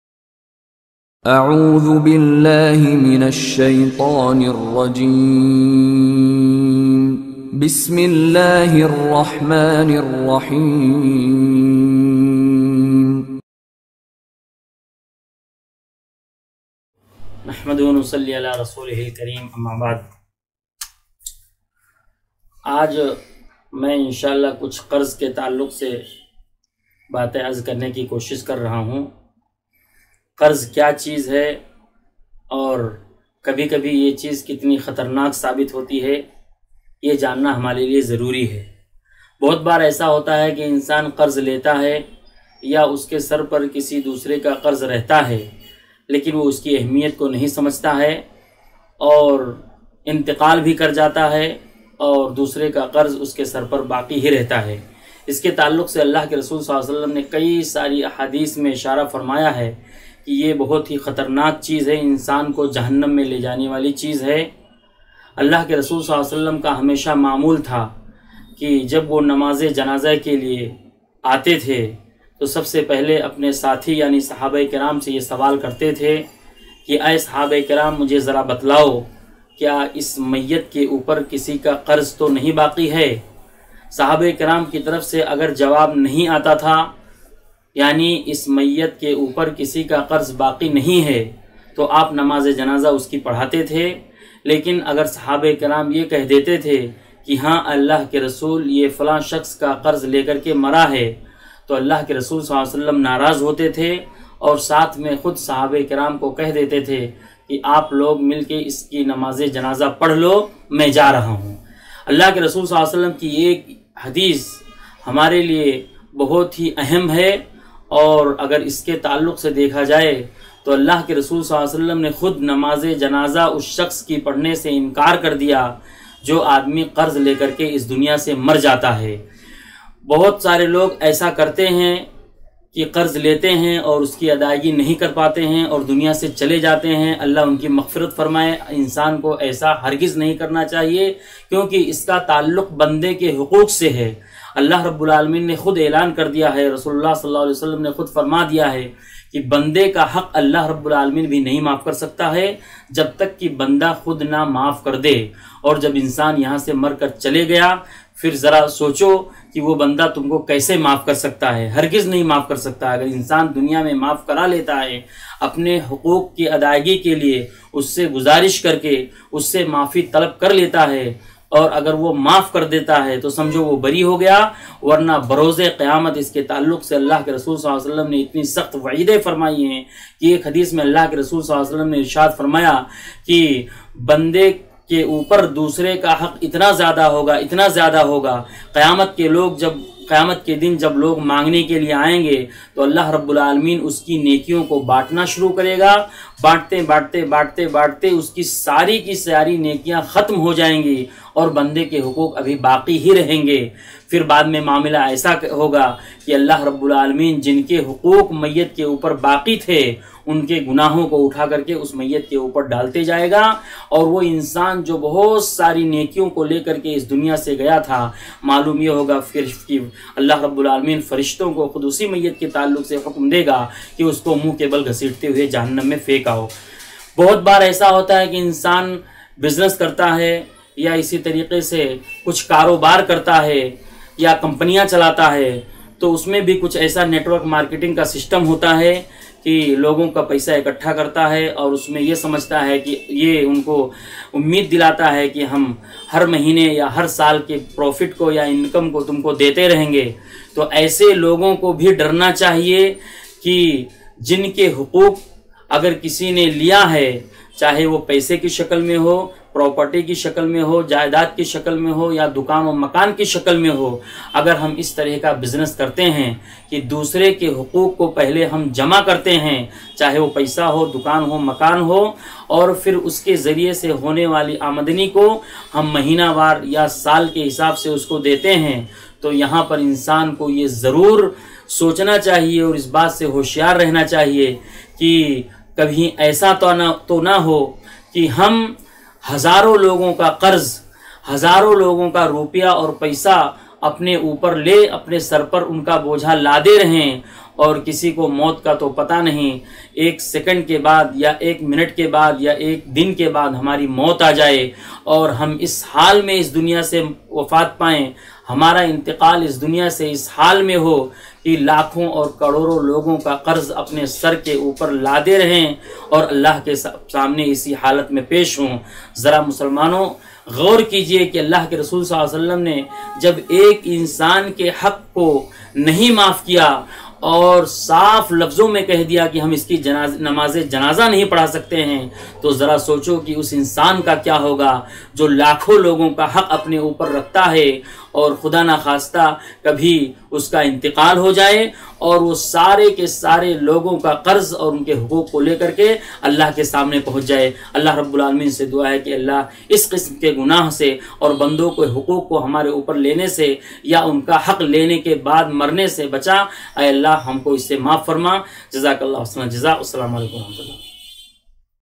Shaikh Shakeel Sanabili on Karz (Loan) and Companies that take in Investments
Recorded on May 5, 2019 - Mumbai.  Lecture on Loan and companies that take in investments, Muamalaat by Shaikh Shakeel Sanabili.